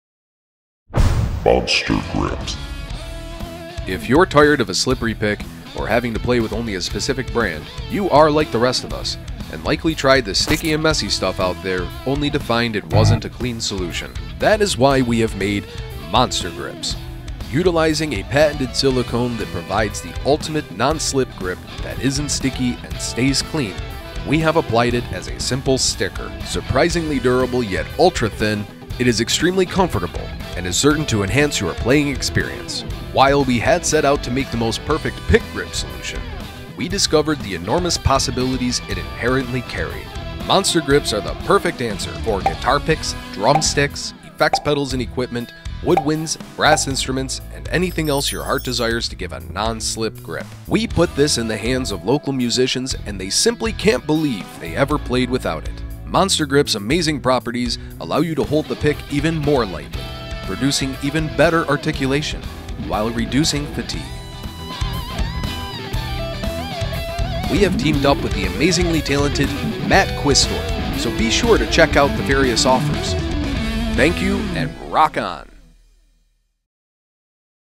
Monster Grips™: The Ultimate Grip for Guitar Picks and More!
Put an end to slippery guitar picks!

Monster Grips™ are a revolutionary guitar pick grip that is super grippy, non-sticky, and stays clean. Surprisingly durable, yet ultra-thin, it is extremely comfortable and is certain to enhance your playing experience. Monster Grips™ amazing properties allow you to hold the pick even more lightly, producing even better articulation, while reducing fatigue.

Monster Grips LLC